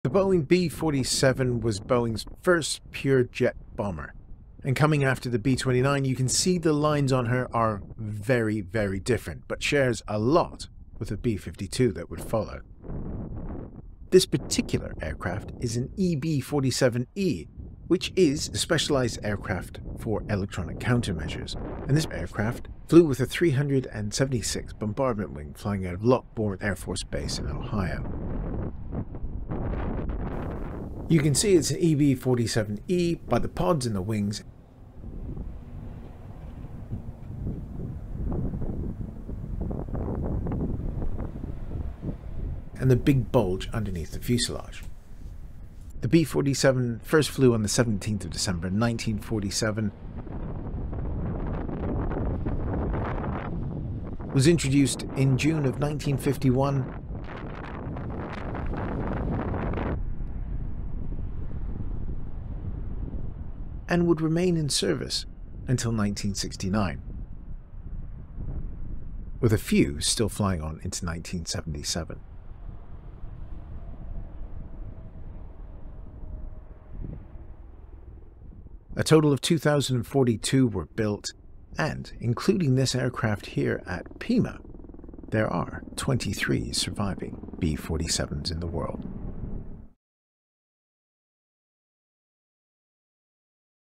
Boeing EB-47E walkaround at the Pima Air and Space Museum
The B-47 Stratojet was Boeing's leap into the Jetage. The B-47 would be the USAF's Strategic Air Command's frontline bomber until the arrival of the B-52, but it would also see service in the reconnaissance, ELINT and weather roles. Coupled with that, she looks fantastic!

To learn more about Pima's EB-47E, you can check out the aircraft's page on the Pima

Find out who is in the Arizona Aviation Hall of Fame
Want to know how the Tucson Military Vehicle Museum is progressing?